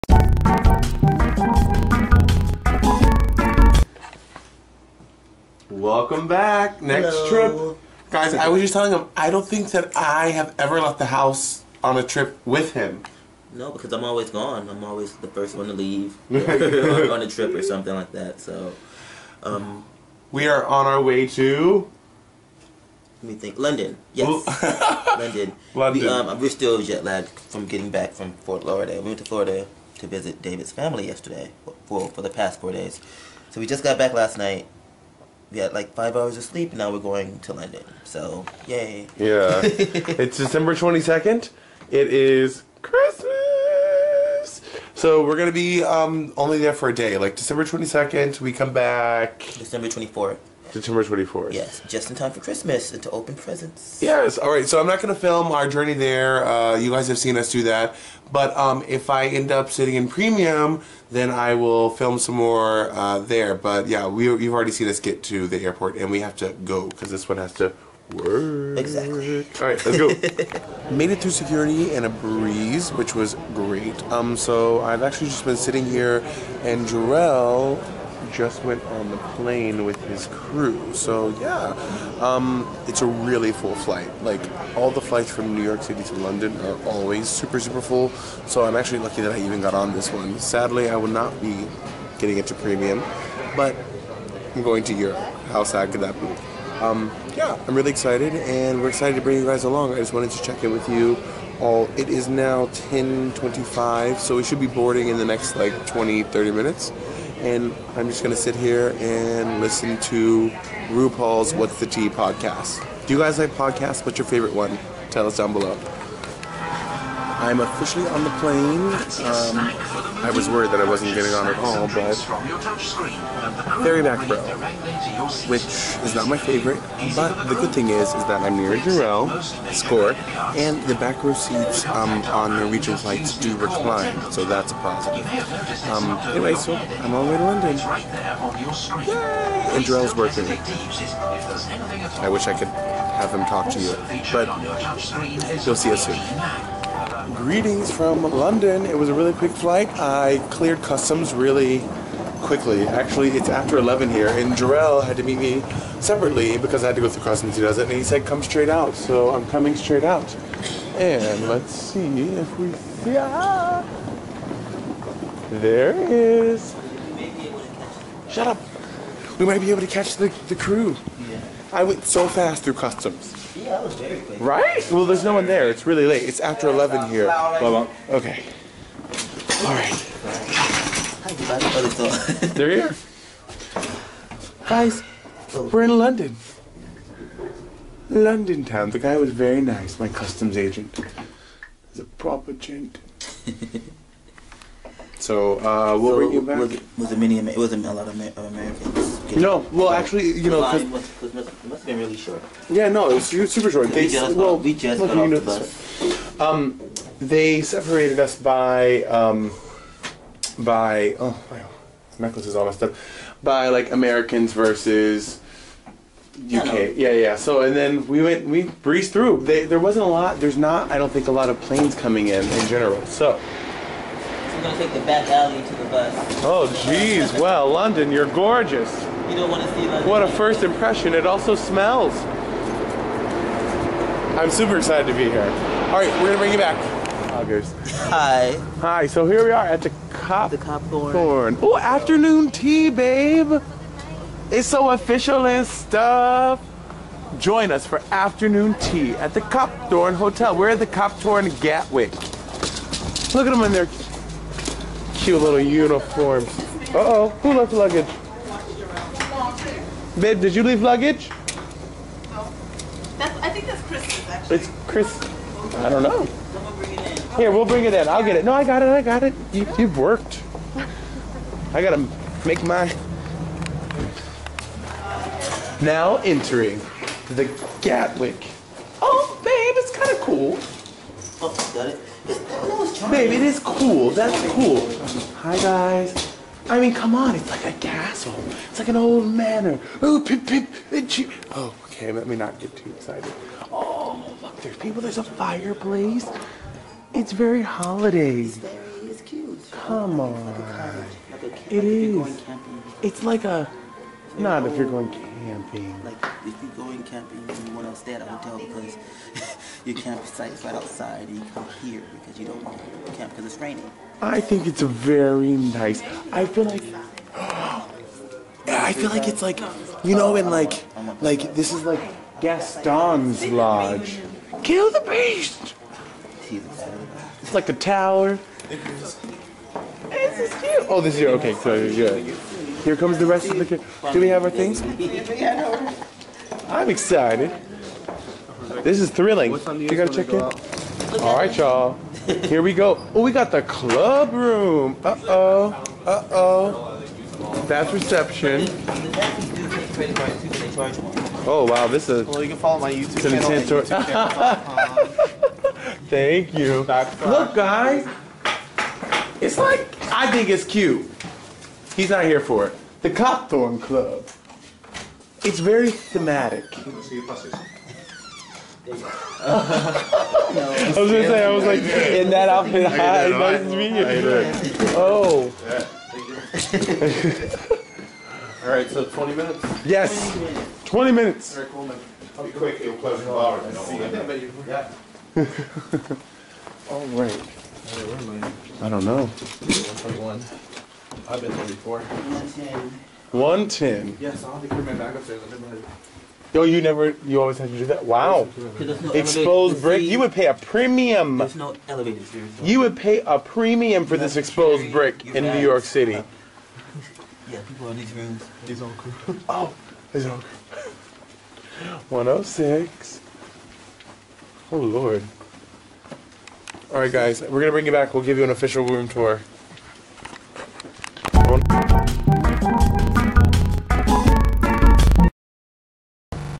Traveling From NYC To London!
The journey begins! Travel with the boos from NYC to London!!